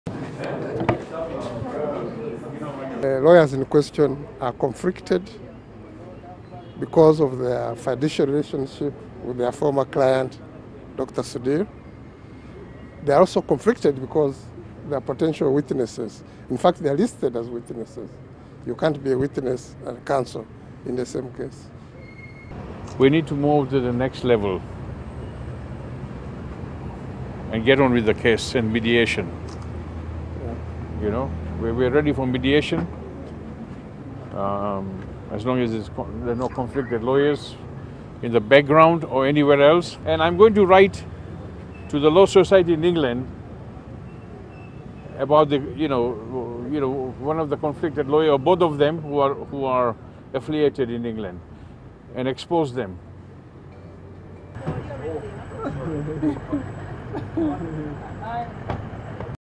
BOU lawyers thrown out as businessman Sudhir Ruparelia wins round one in Crane bank case
The head of the commercial Court, justice David Wangutsi disqualified lawyers Timothy Masembe Kanyerezi and David Mpanga from representing Bank of Uganda over conflict of interest having conceded to being  Sudhir Ruparelia' s lawyers for 12 years.

In his ruling read to court by deputy registrar Lillian Buchana , justice  Wangutusi  said the lawyers might be required  as potential witnesses to settle the current dispute  between Bank of Uganda and  Sudhir Ruparelia for being   involved in negotiations ,  drafting and witnessing of agreements before  Bank of Uganda took over Crane Bank in October 2016.

However the 2 lawyers; Masembe Kanyerezi and David Mpanga t sent  a representative  David Adrinko who  informed court of their intentions to appeal against  Justice Wangutusi' s ruling.